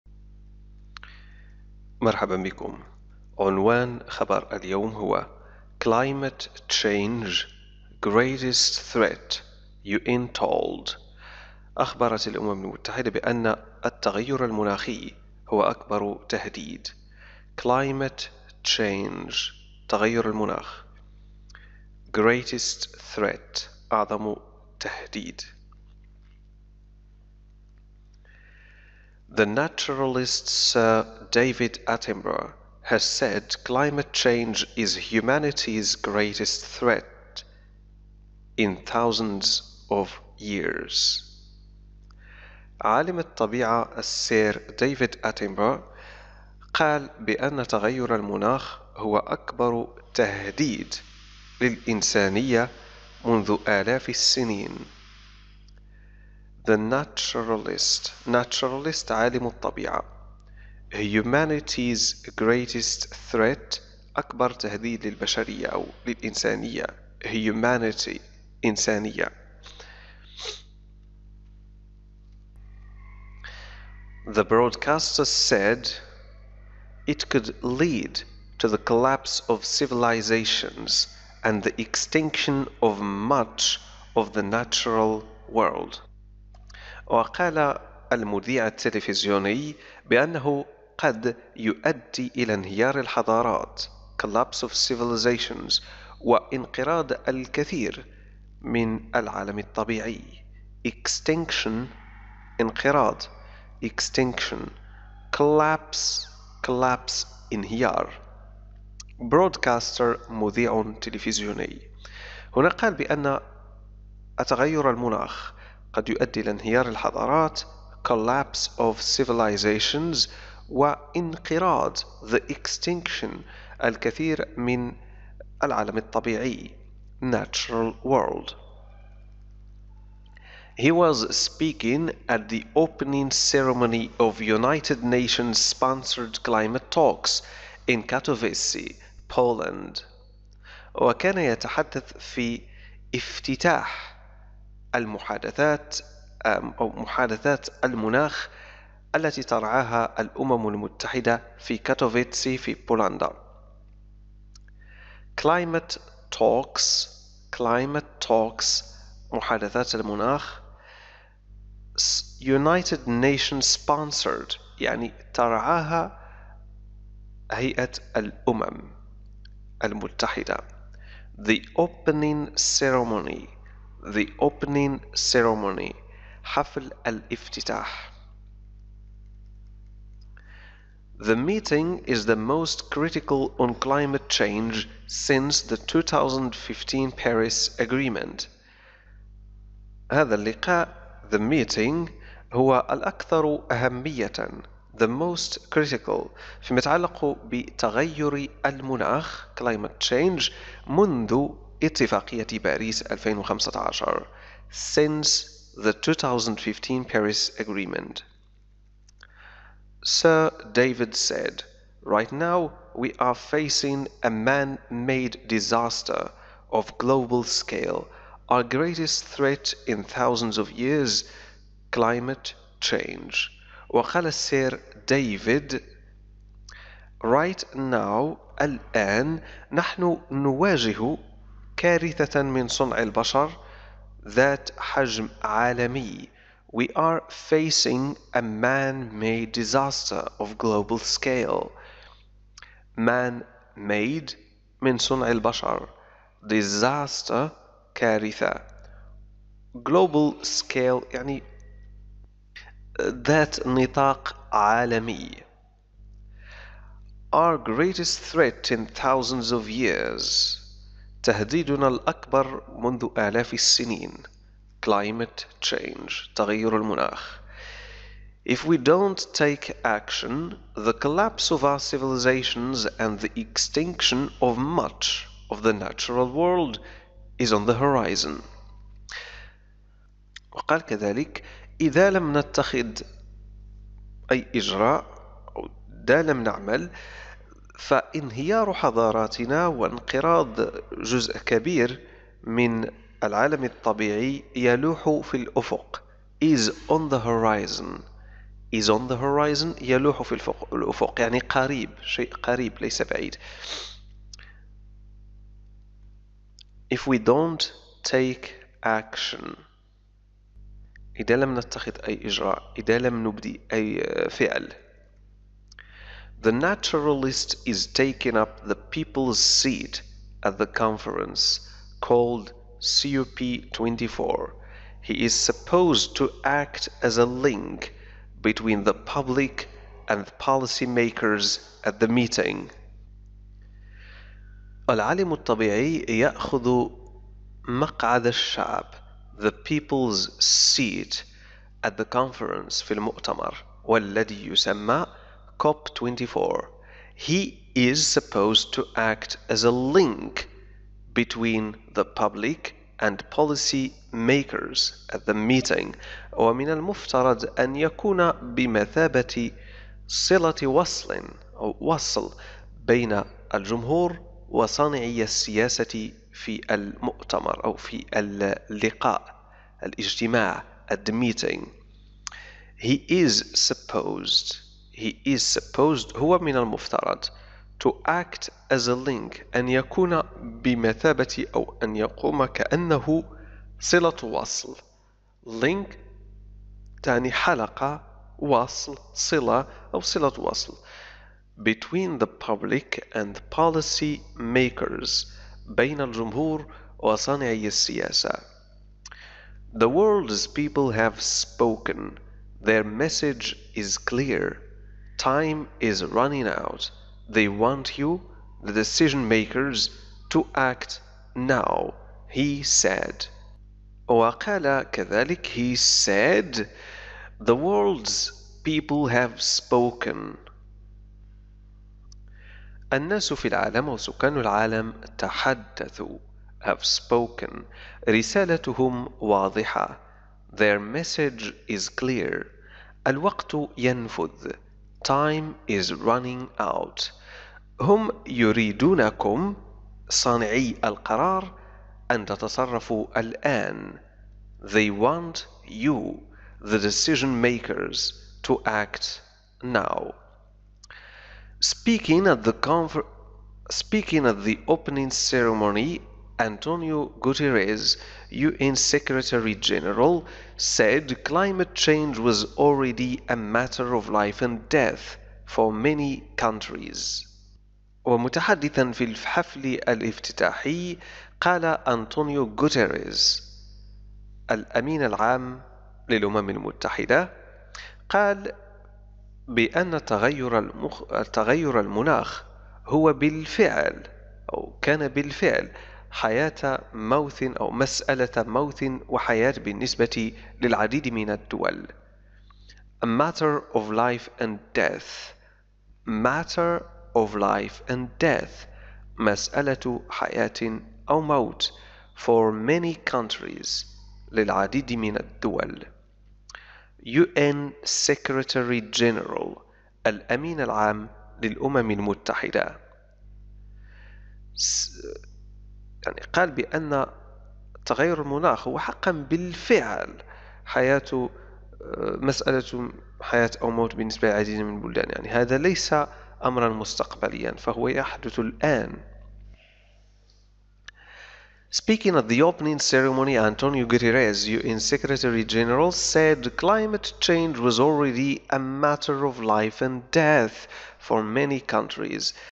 تعلم الانجليزية موضوع تغير المناخ تهديد خطير للبشرية
The naturalist Sir David Attenborough has said climate change is humanity's greatest threat in thousands of years.

The broadcaster said it could lead to the collapse of civilisations and the extinction of "much of the natural world".

He was speaking at the opening ceremony of United Nations-sponsored climate talks in Katowice, Poland.

The meeting is the most critical on climate change since the 2015 Paris agreement.

Sir David said: "Right now, we are facing a man-made disaster of global scale. Our greatest threat in thousands of years. Climate change.

"If we don't take action, the collapse of our civilisations and the extinction of much of the natural world is on the horizon."

The naturalist is taking up the "People's Seat" at the conference, called COP24. He is supposed to act as a link between the public and policy-makers at the meeting.

"The world's people have spoken. Their message is clear. Time is running out. They want you, the decision-makers, to act now," he said.

Speaking at the opening ceremony, Antonio Guterres, UN Secretary-General, said climate change was already "a matter of life and death" for many countries.